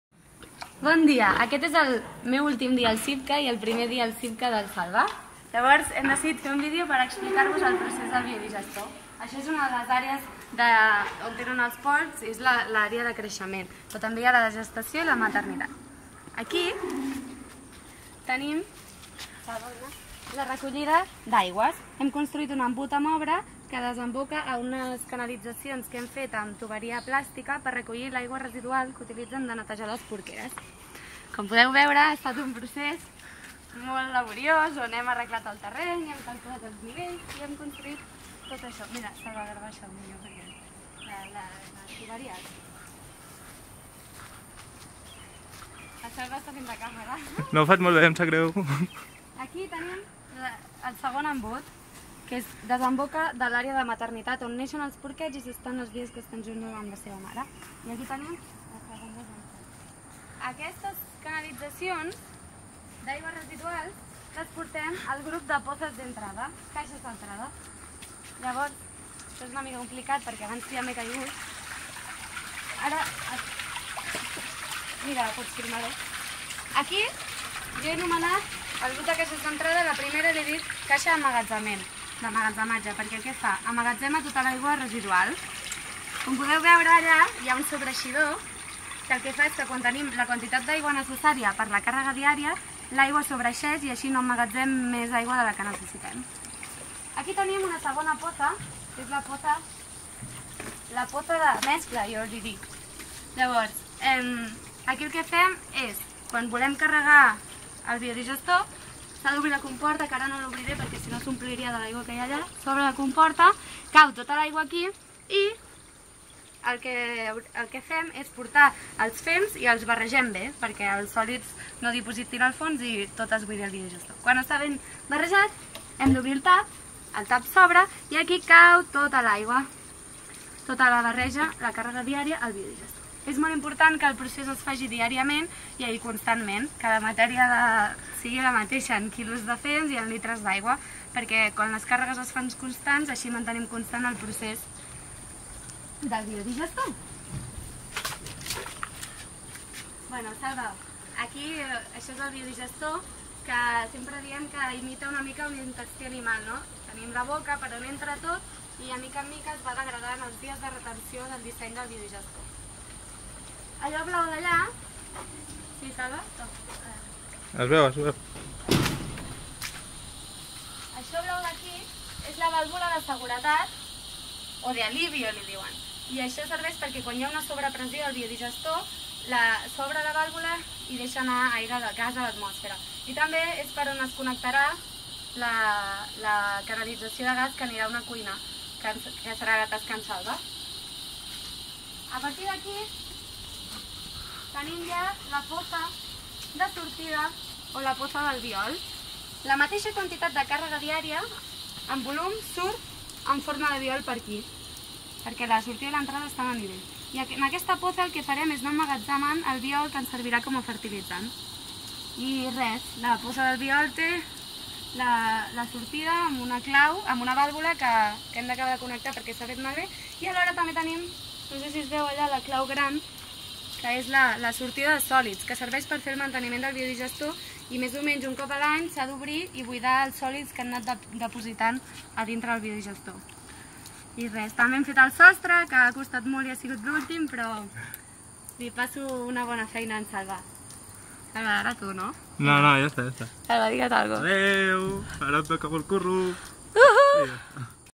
Construïm un biodigestor a l'Equador per promoure les energies renovables
Les voluntàries d’ESF Girona, dins del projecte “Enfortiment de capacitats en el context universitari, comunitari i institucional per a l'aplicació d'energies renovables a la província de Pastaza”, hem recolzat la construcció d’un biodigestor al Centre d’Investigació de la Universidad Estatal Amazónica, a l’Equador.
Aquesta tecnologia permetrà aprofitar els excrements del bestiar per aconseguir biofertilitzant i biogàs per una petita cuina i acabar amb l’abocament d’aigües residuals al riu!
A més, promourà l’estudi d’aquesta i altres energies renovables a l’Amazònia equatoriana.
Vols estar al dia de les activitats del Grup Girona d'ESF? Segueix el seu Facebook